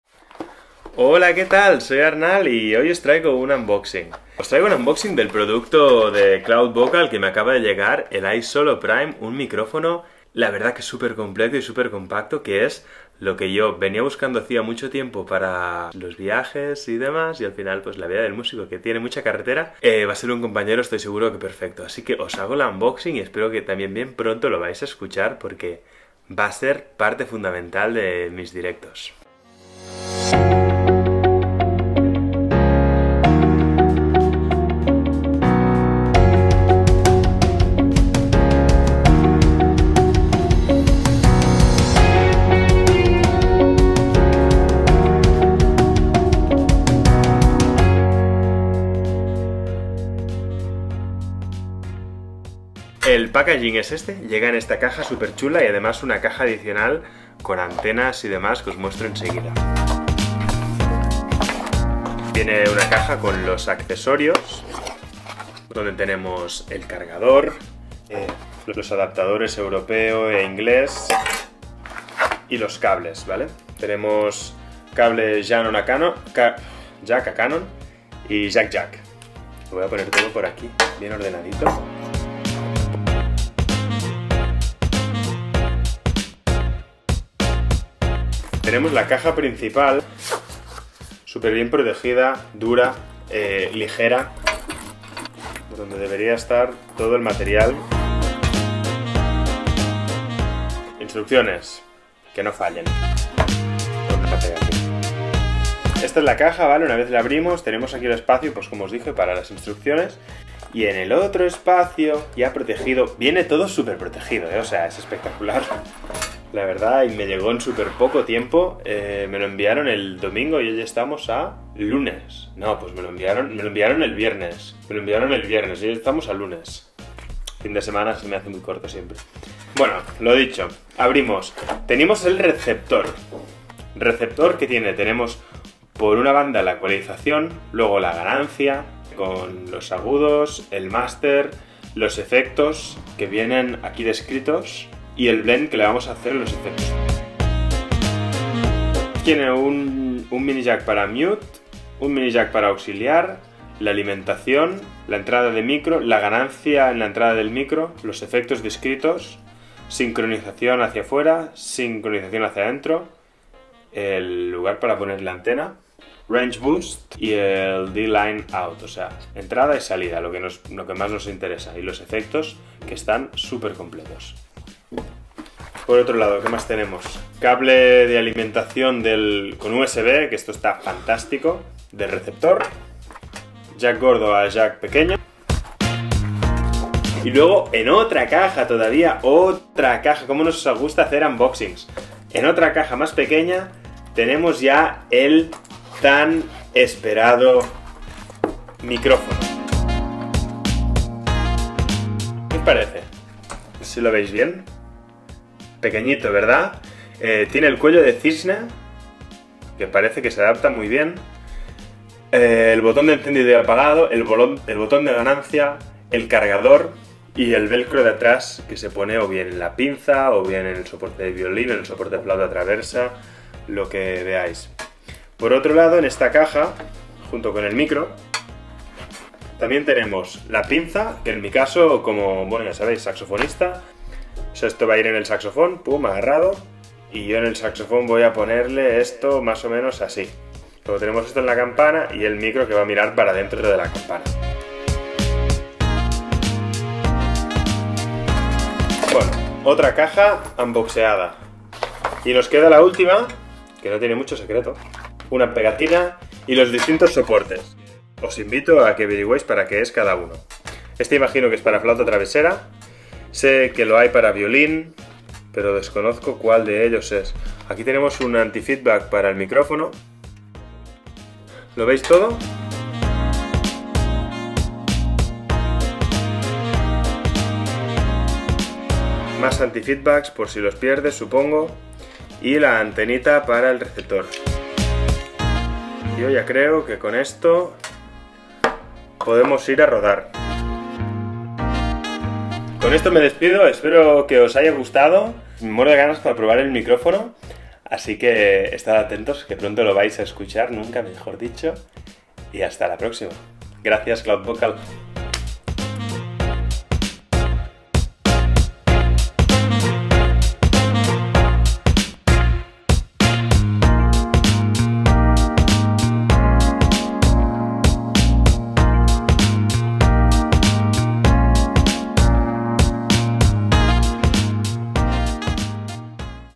ISOLO Prime Unboxing | Microphone Wireless System by CLOUDVOCAL
I just recieved the ISOLO Prime, the top wireless system by @cloudvocal-isolowirelessin5096, a small but super powerful wireless microphone that will be my best partner for my live performances on saxophone.

The ISOLO comes with built-in effects that I'm sure you will love.